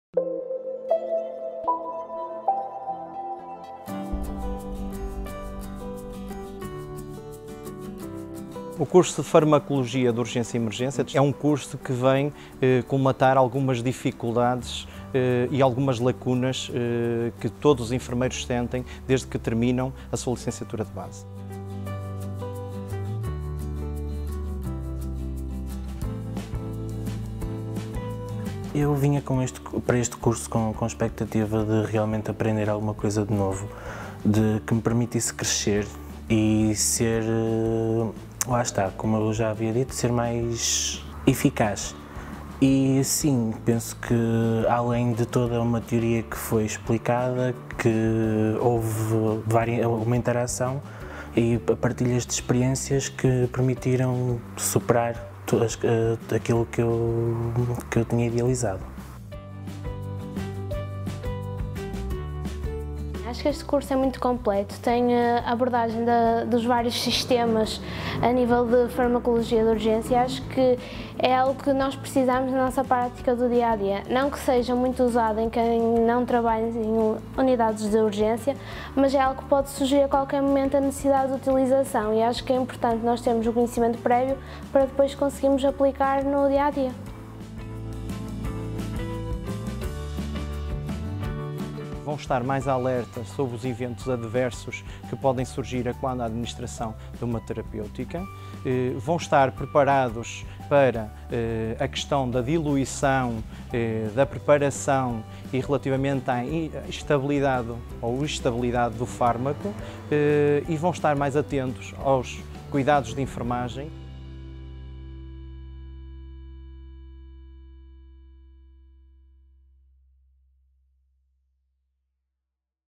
Farmacologia de Urgência e Emergência para Enfermeiros: o curso | Bwizer
No curso de Farmacologia de Urgência e Emergência para Enfermeiros irás conhecer os fármacos mais utilizados em situações de urgência e emergência.

Aprenderás a utilizá-los, adquirirás conhecimentos sobre as situações clínicas que podem contraindicar o seu uso e será capaz de identificar as interações farmacológicas mais relevantes em contexto de urgência e emergência.

Uma intervenção rápida e segura é algo importante em todas as áreas da saúde, no entanto, neste contexto não é apenas importante, mas sim imperioso!